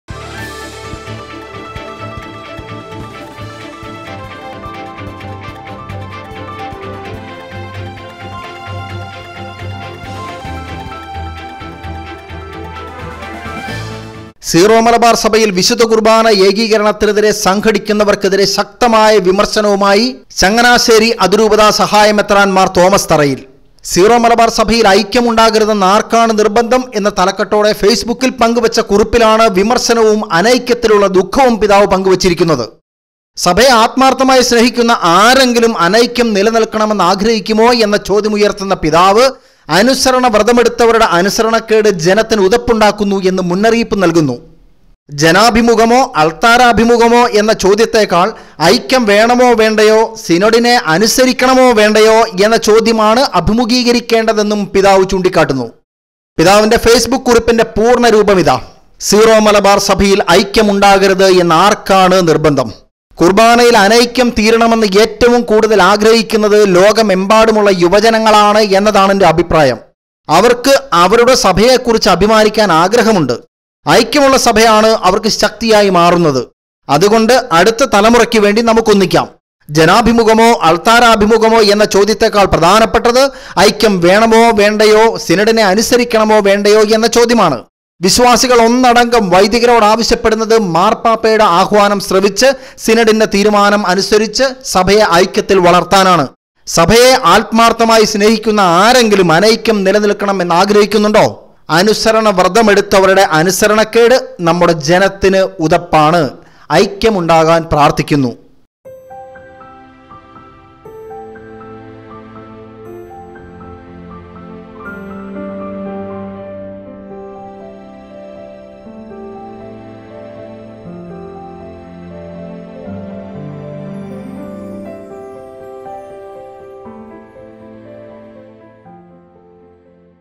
ഐക്യം ഉണ്ടാകരുതെന്ന് ആര്‍ക്കാണ് നിര്‍ബന്ധം. 'സീറോ മലബാര്‍ സഭയില്‍'..ബിഷപ്പ് മാര്‍ തോമസ് തറയില്‍...!
അനുസരണവ്രതമെടുത്തവരുടെ അനുസരണക്കേടിനെതിരെ
ബിഷപ്പ് മാര്‍ തോമസ് തറയില്‍...!!!

'സീറോ മലബാര്‍ സഭയില്‍' ഐക്യം ഉണ്ടാകരുതെന്ന് ആര്‍ക്കാണ് നിര്‍ബന്ധം..?